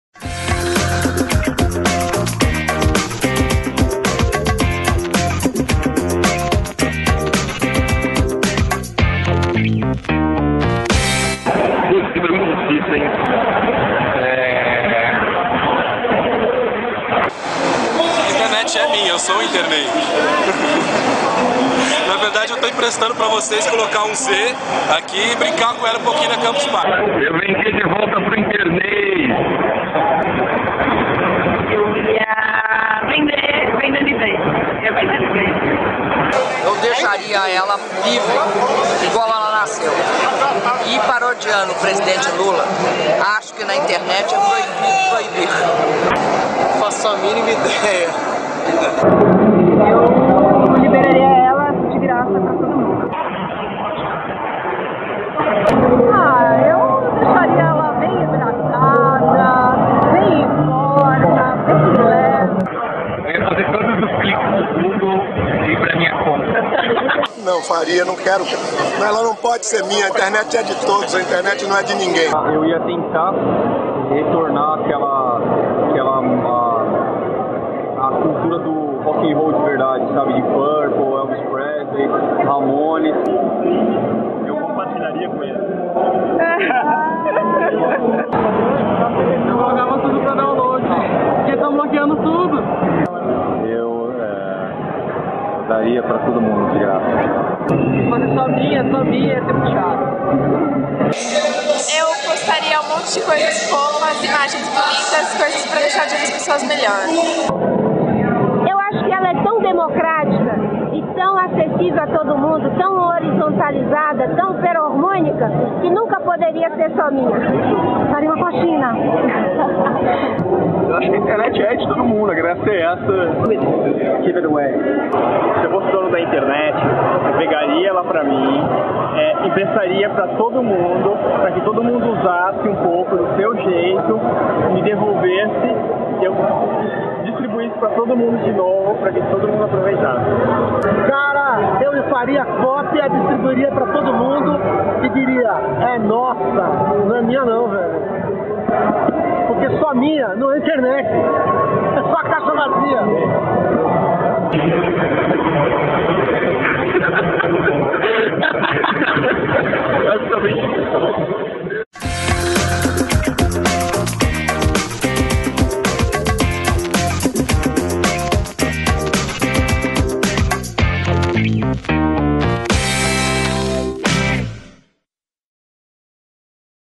OS DONOS DA INTERNET
Imaginando, apenas por um dia, que o Google não fosse dono da internet inteira e que ela fosse inteirinha sua, o que você faria com ela? Foi essa pergunta que fizemos pra várias pessoas, anônimas e famosas, que circularam pela Campus Party 2010. Em um evento onde só se fala em internet livre, o que fariam OS DONOS DA INTERNET?

FILMAGENS (ultra caseiras) - Bia Granja, Gabriela Motta, Thaís Aux